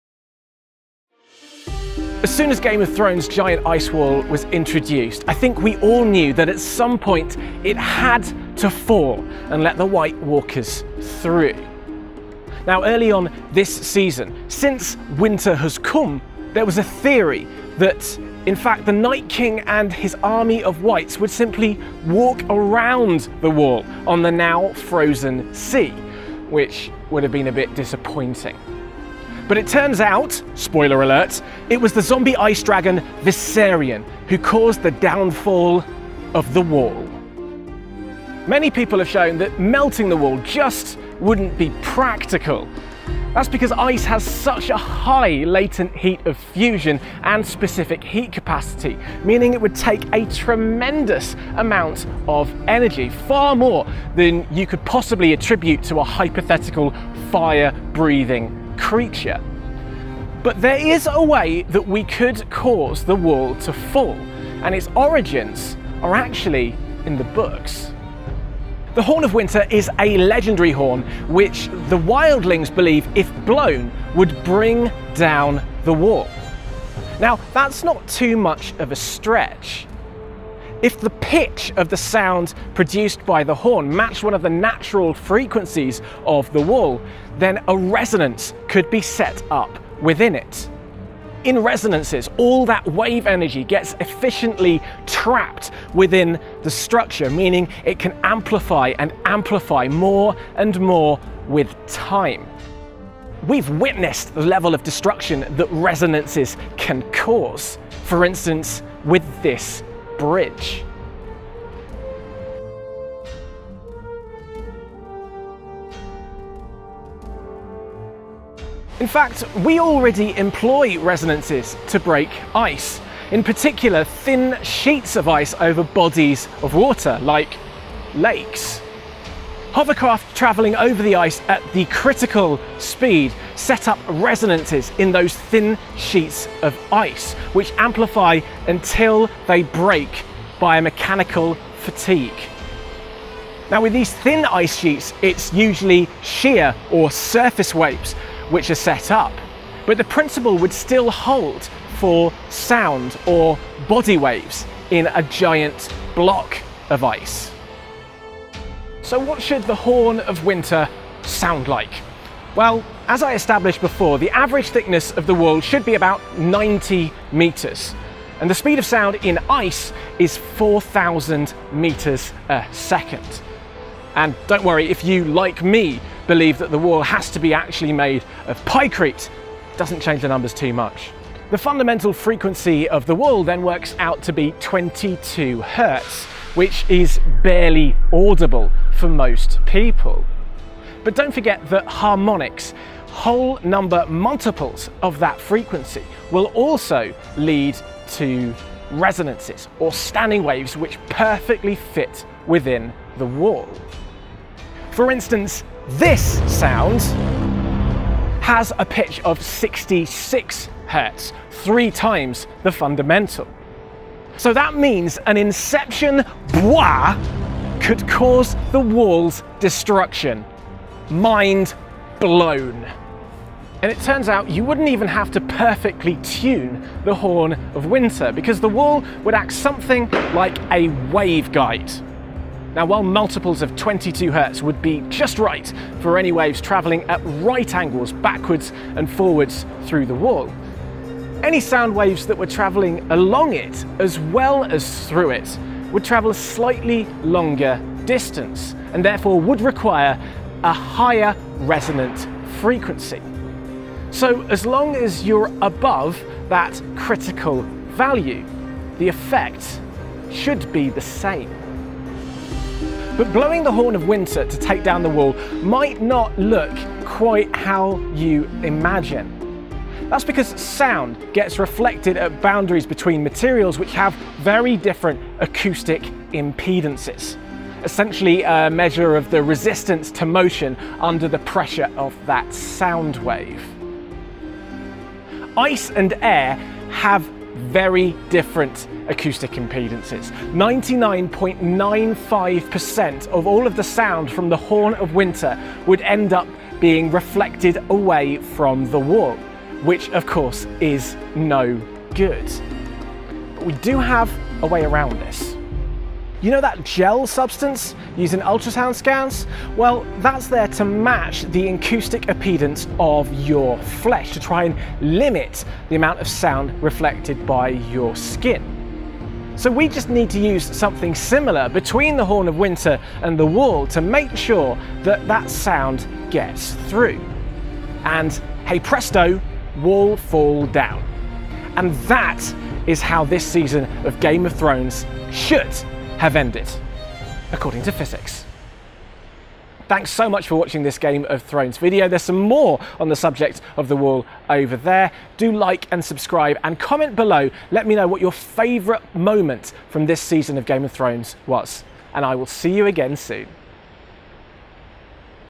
How Game of Thrones' Wall Should Really Have Fallen | The Horn of Winter | Physics vs Film (and TV)
Game of Thrones giant ice wall was always going to fall at some point, but what is the way that would make most sense from a Physics point of view? Turns out the answer was in the books all the time... the Horn of Winter! SPOILER FREE AFTER 01:01

00:38 The problem with melting The Wall
01:08 The Horn of Winter and Resonances
01:58 How to break ice with resonances
02:38 What should the Horn of Winter sound like?
04:22 What would it look like?

Filmed at The London Wall